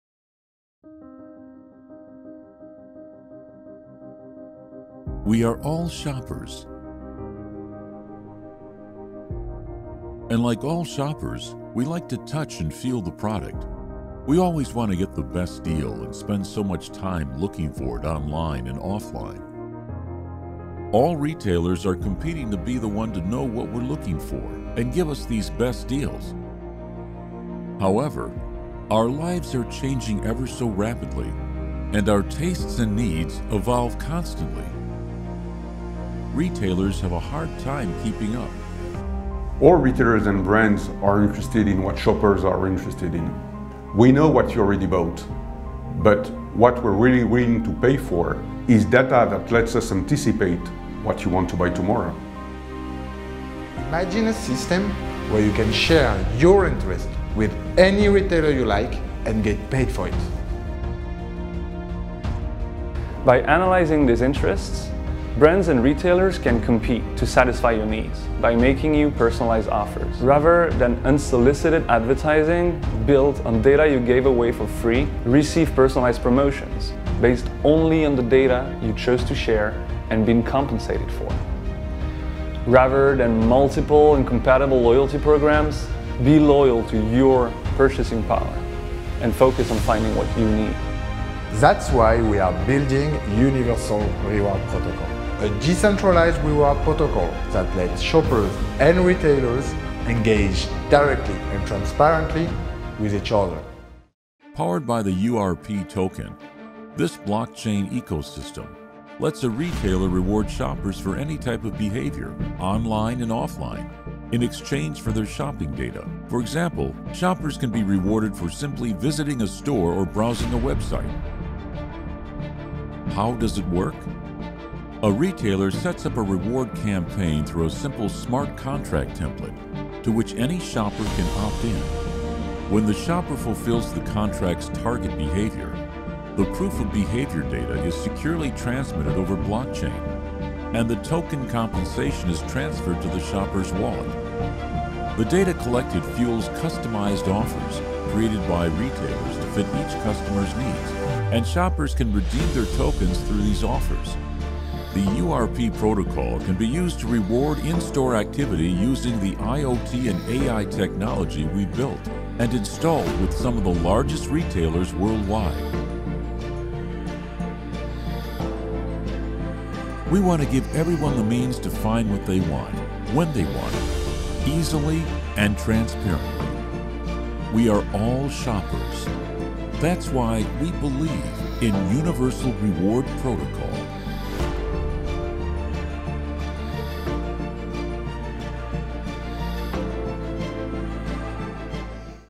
Universal Reward Protocol - Introduction
Universal Reward Protocol is a blockchain-based protocol where retailers can reward shoppers for multiple behaviors like simply visiting the store. By accepting to share their shopping data with the network, a shopper earns reward tokens, while the data is used by retailers to build redemption offers that best suit each customer’s needs. Shoppers can redeem their tokens through these exclusive and personalized offers.